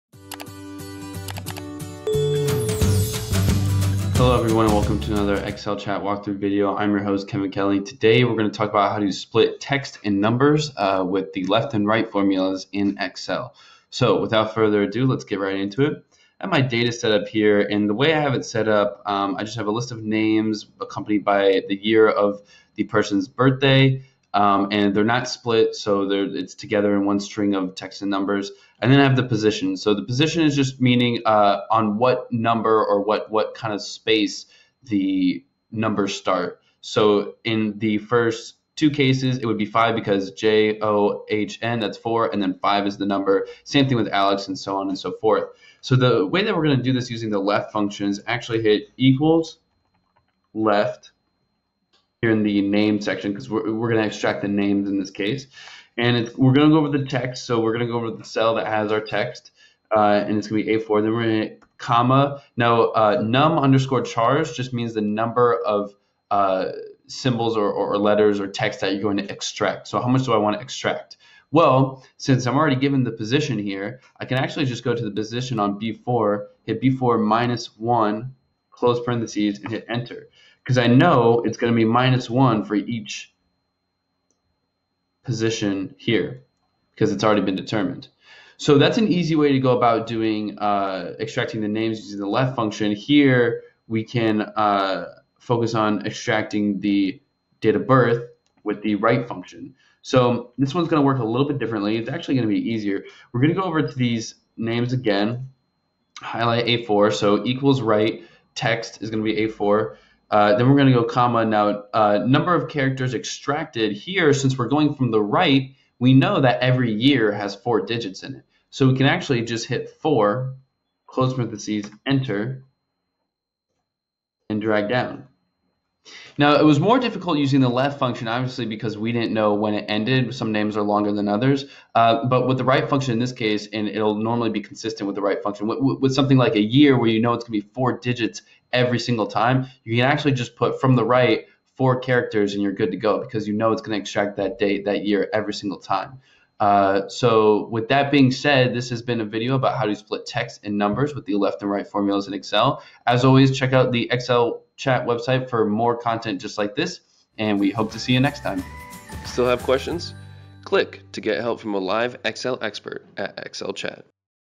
How to Split Text and Numbers with a Formula in Excel
To split text and numbers, you can use a formula based on the FIND function, the MIN function, and the LEN function with the LEFT or RIGHT function, depending on whether you want to extract the text or the number.